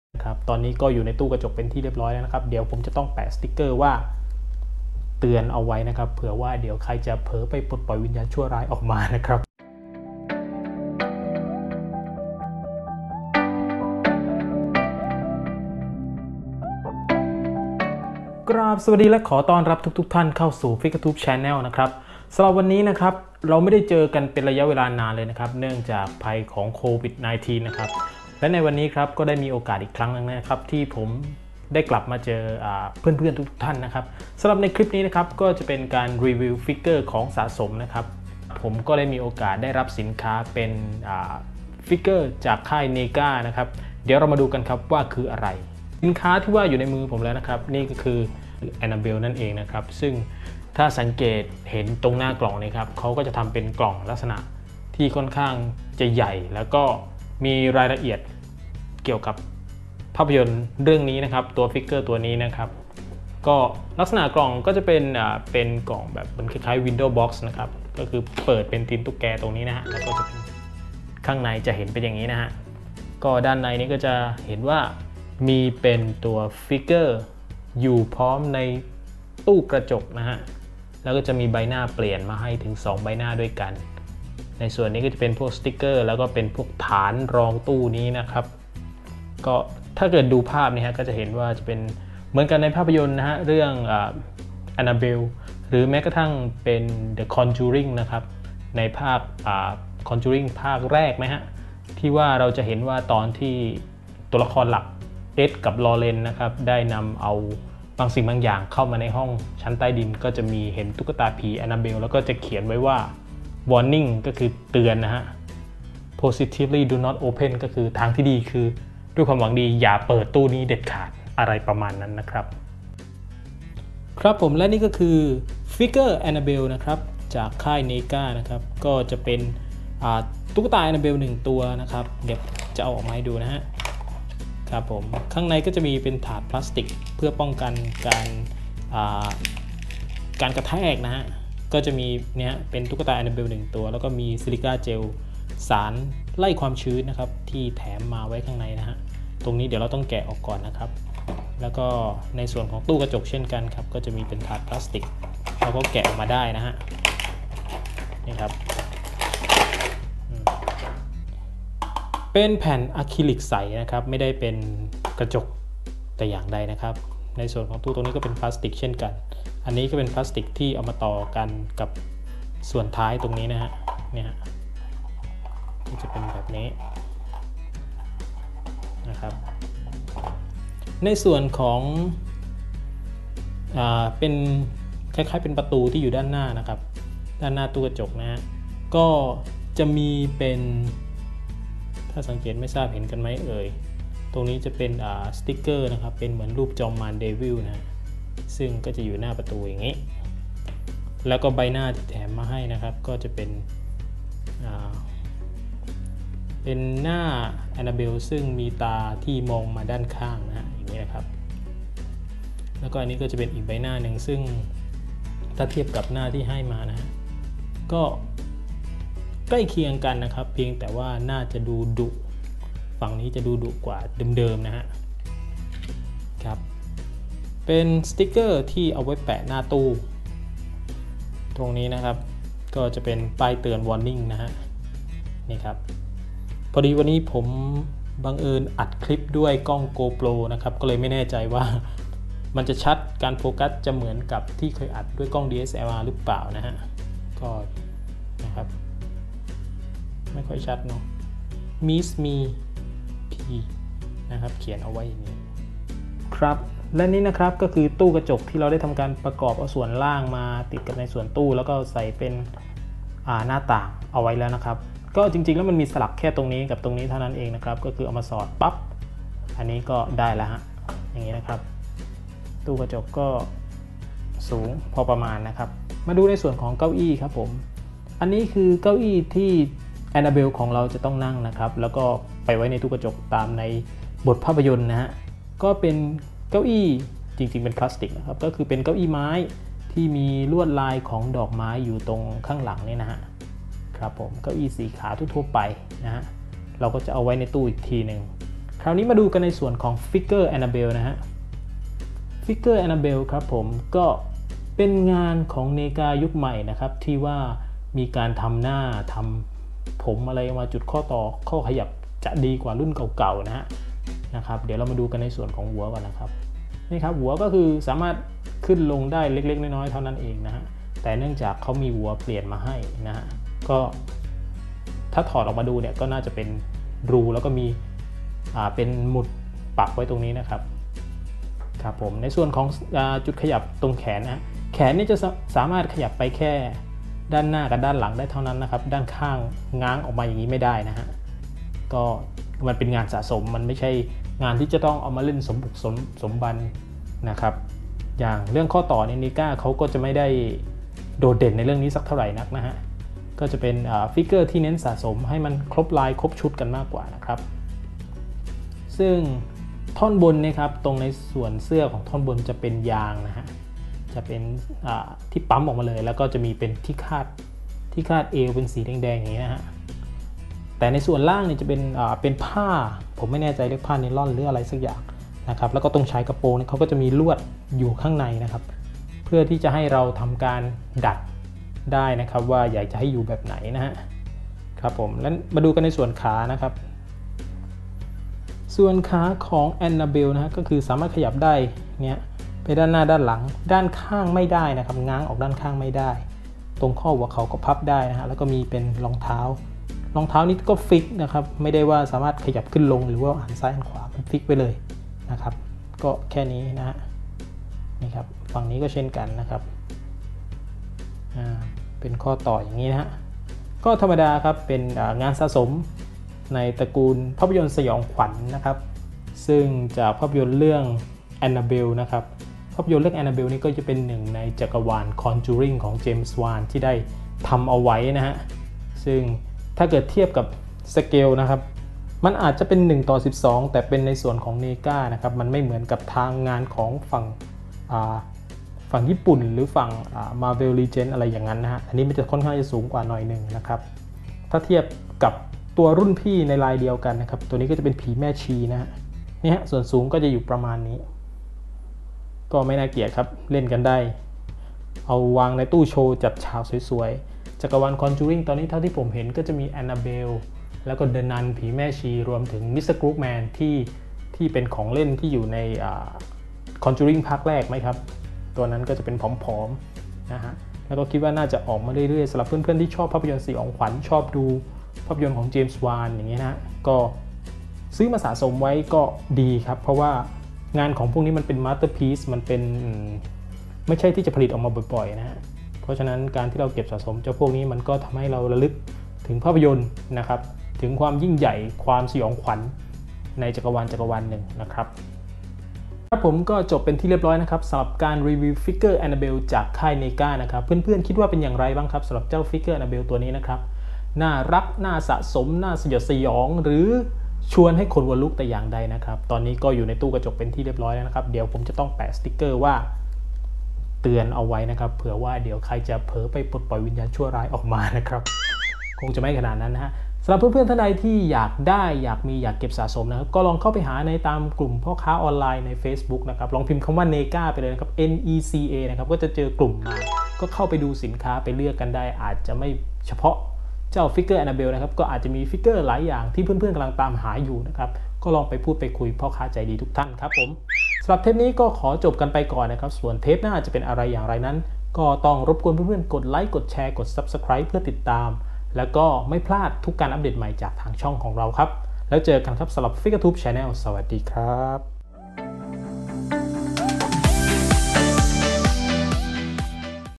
Annabelle งานสะสมจากค่าย Neca
งานสะสมอีกชิ้นหนึ่ง จากค่าย Neca ตัวละครหลักจากภาพยนต์ AnnaBelle Comes Home